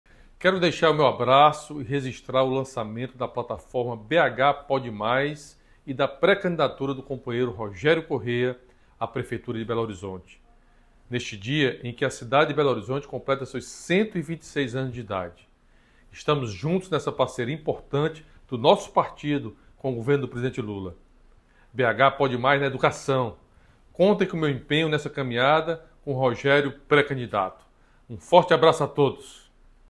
BH PODE MAIS NA EDUCAÇÃO 🎒 📖
Outra liderança do governo Lula, o ministro da Educação, Camilo Santana, também apoia o Movimento BH Pode Mais!